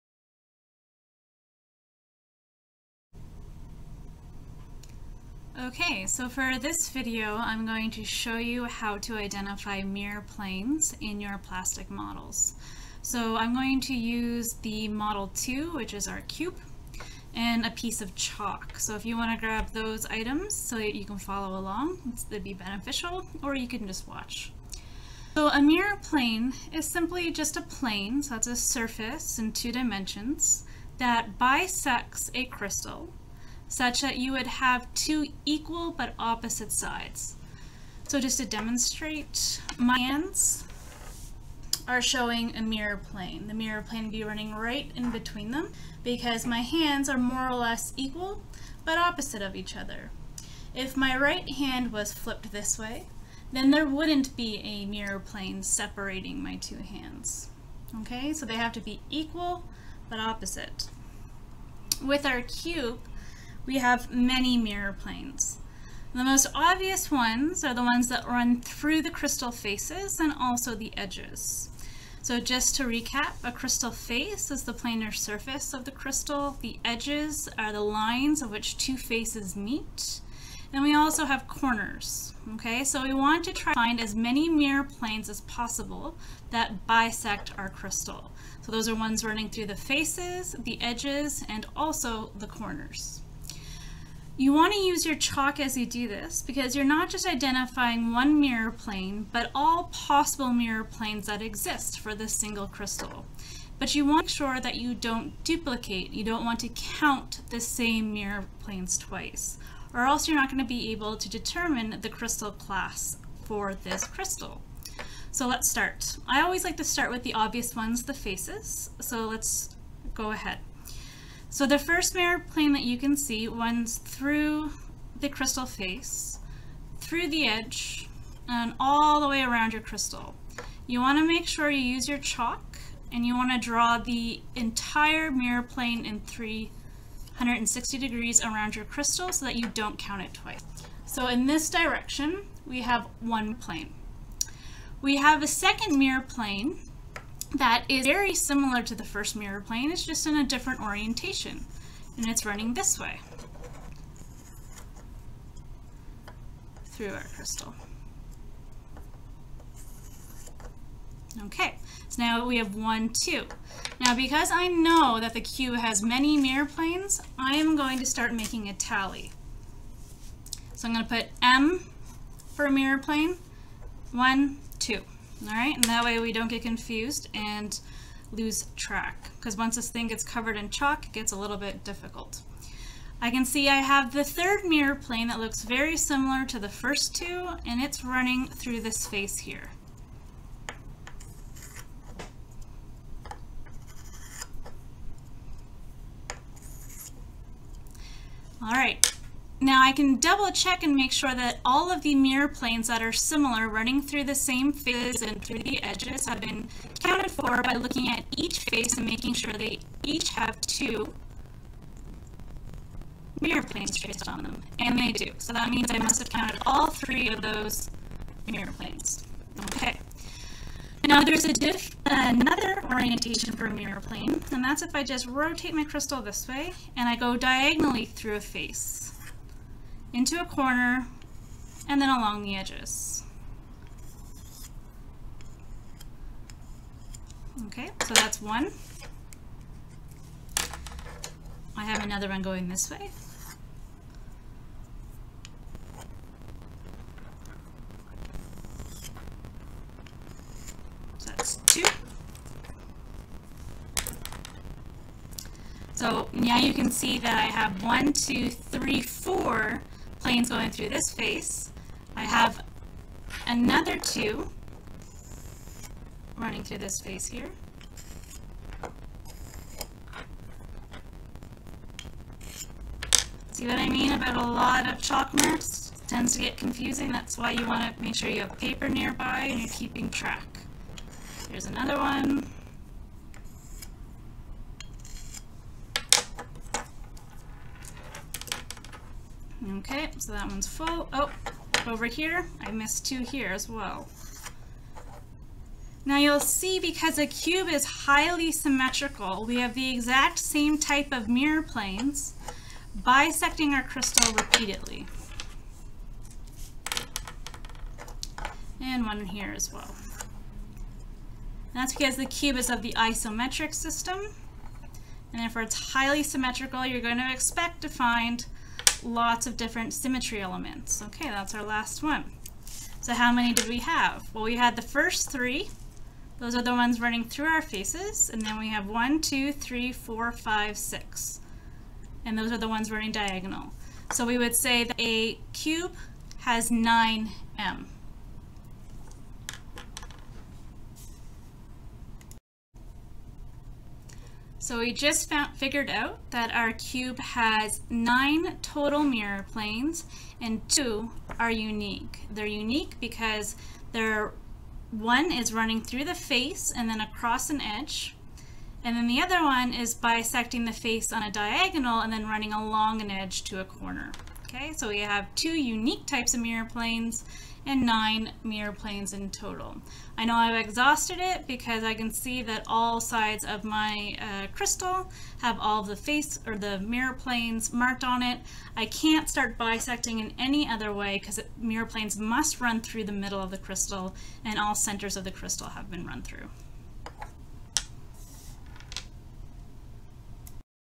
Mineralogy, Crystallography: Point Symmetry, Mirror Planes
A video demonstration intended to help 2nd year Mineralogy students identify Mirror Planes